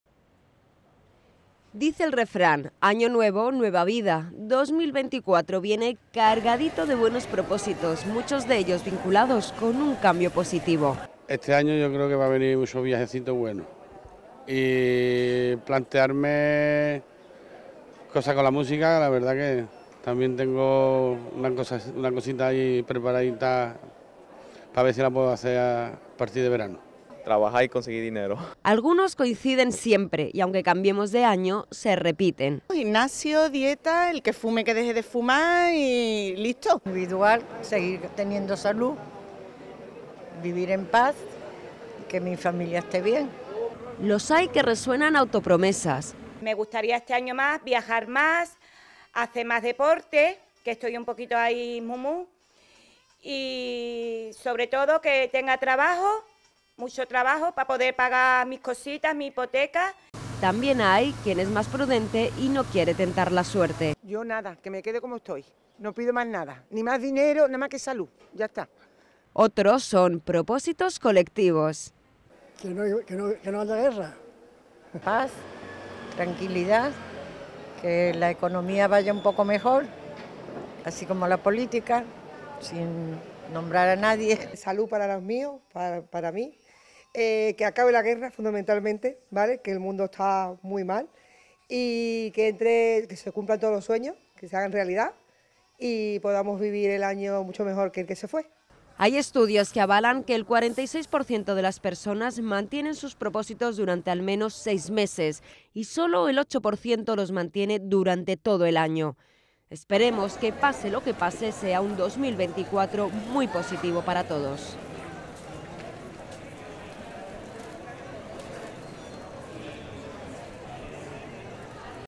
¿Qué le pedimos a este 2024, que acaba de comenzar?
Estrenamos año y con el nuevo comienzo de este 2024 son muchos los propósitos que nos marcamos. Todos encaminados a generar un cambio positivo en nuestros vidas. Estos son algunos de ellos.